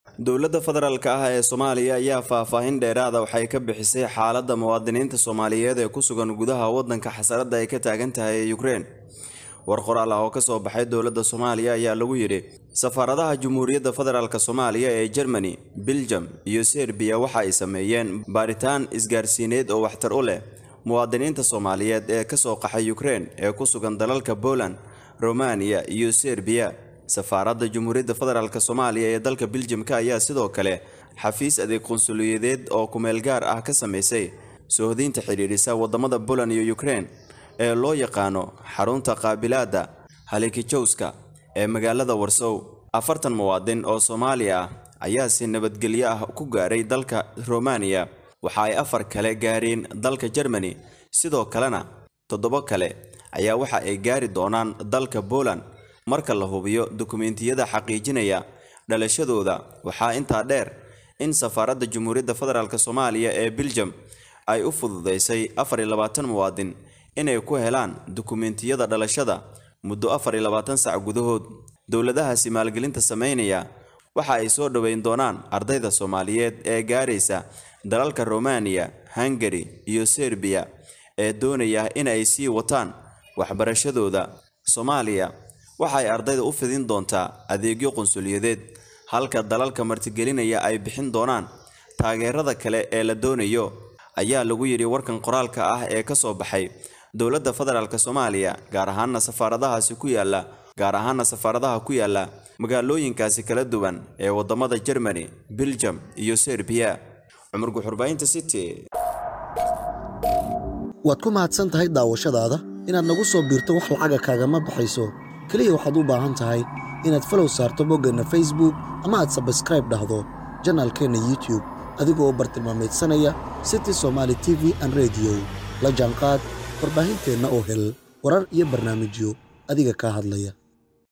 "Afartan (40) muwaadin oo Soomaali ah ayaa si nabadgelyo ah ku gaaray dalka Romania, waxa.."
"Afartan (40) muwaadin oo Soomaali ah ayaa si nabadgelyo ah ku gaaray dalka Romania, waxa ay afar (4) kale ay gaareen dalka Germany, sidoo kale todobo (7) kale waxa ay gaari doonaan dalka Poland" Dowalda Soomaaliya